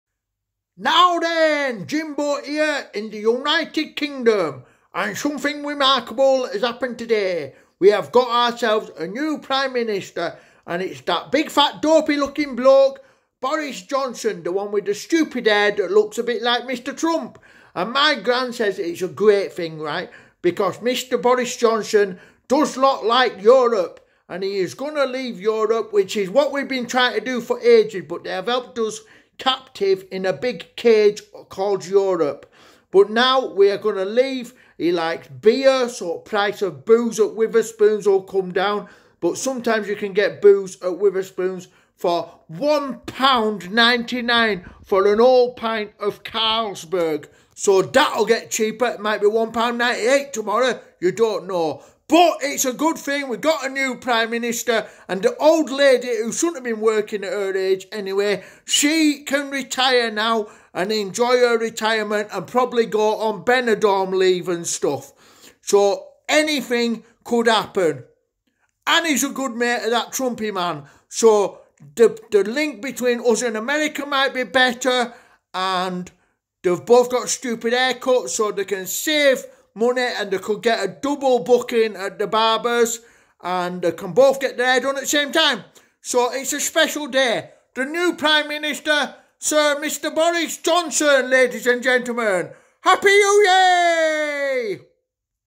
Boris Johnson is the new prime minister of UK with jimbo
Boz is the new prime minister
Boris Johnson is the new prime minister and things are going to be great lol